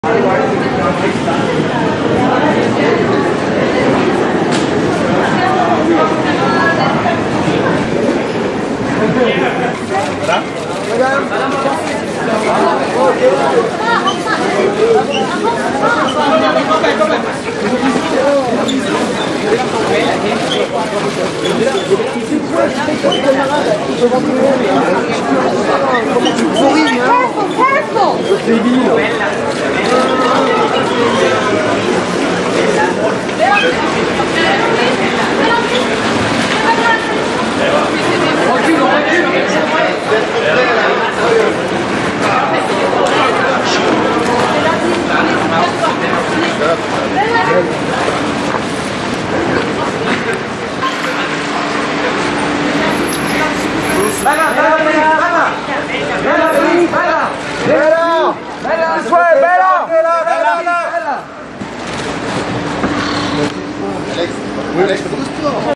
Model Bella Hadid arriving at Cannes airport
Model Bella Hadid arriving at Cannes airport  to attend the 2017 Cannes Film Festival

Cannes, France 16th May 2017